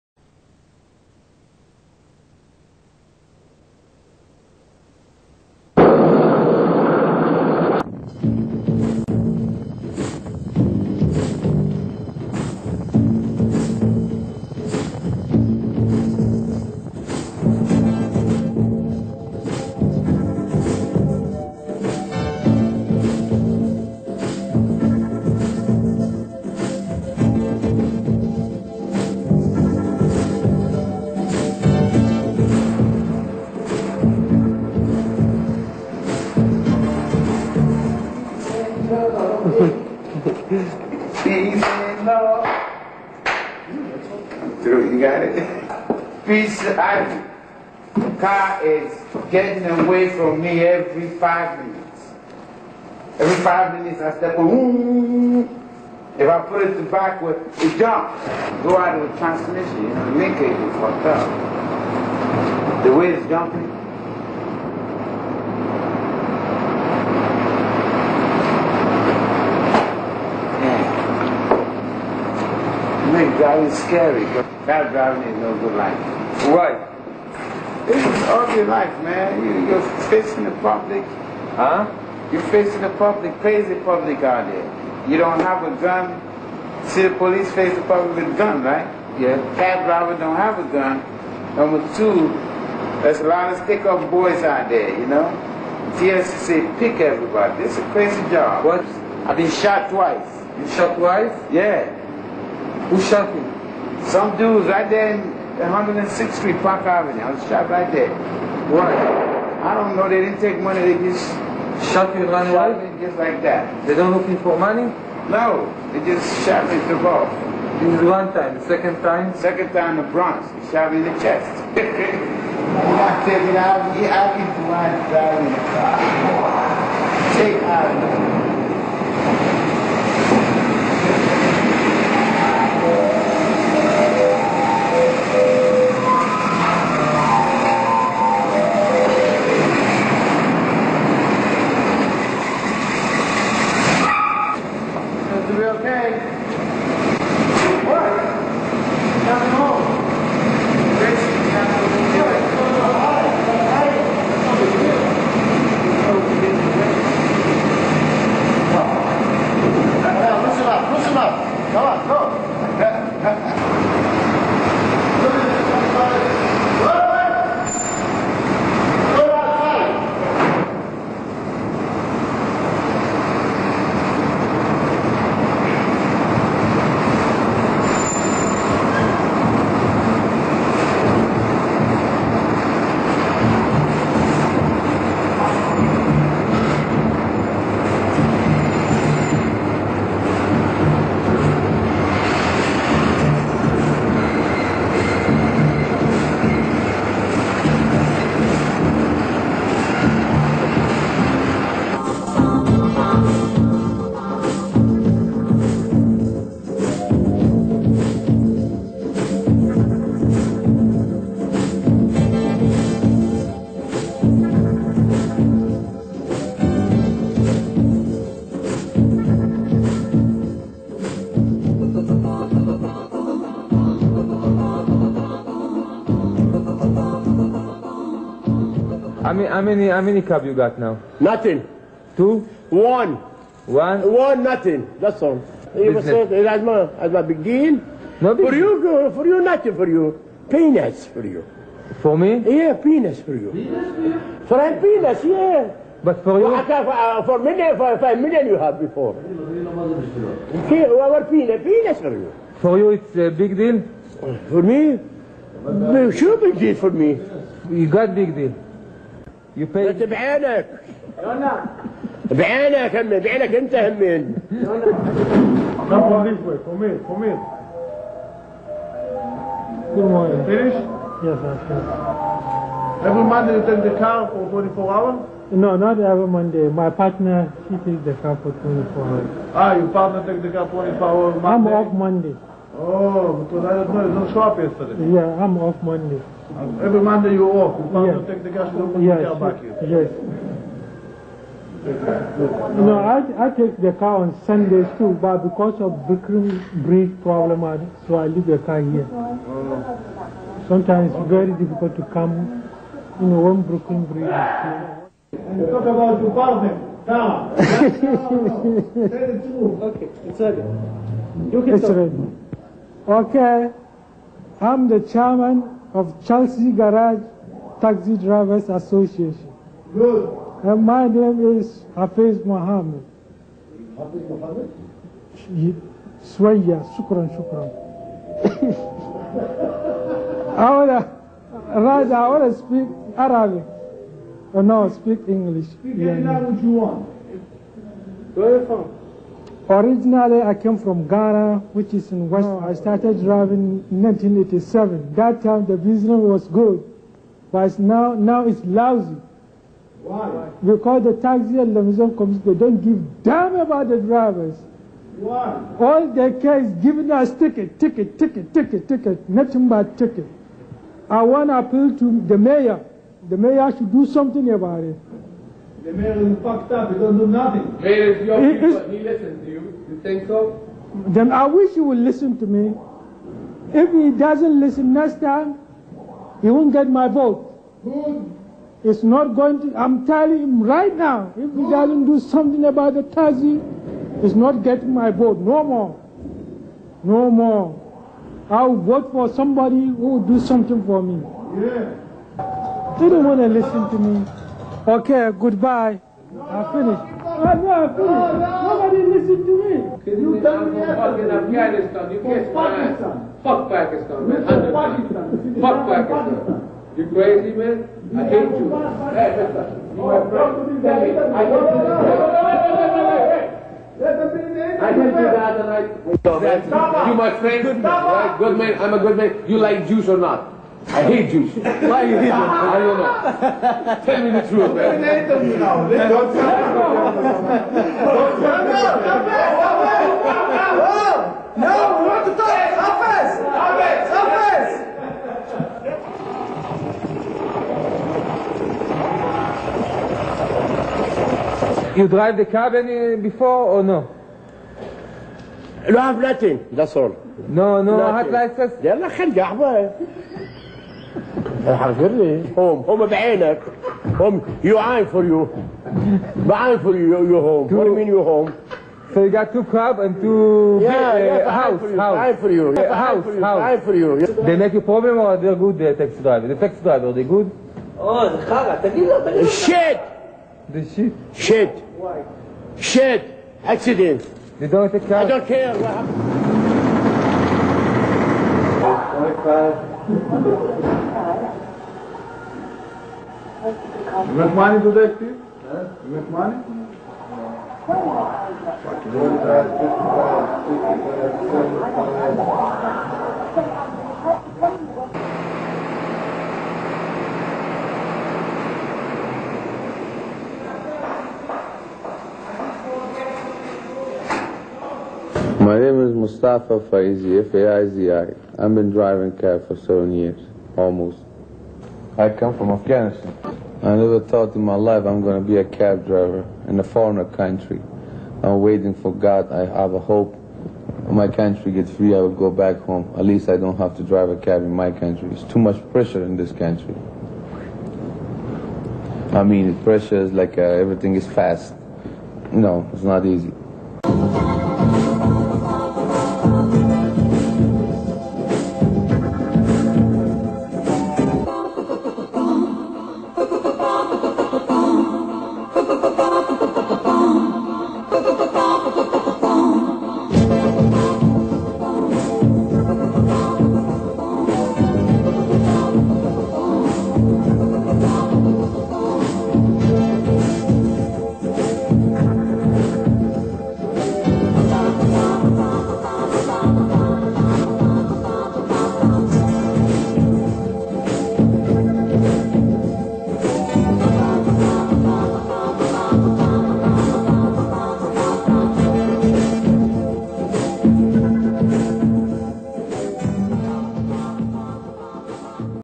TAXI
New York City taxi drivers documentary. Inside look of taxi operation in the big apple. Documented on video 1991.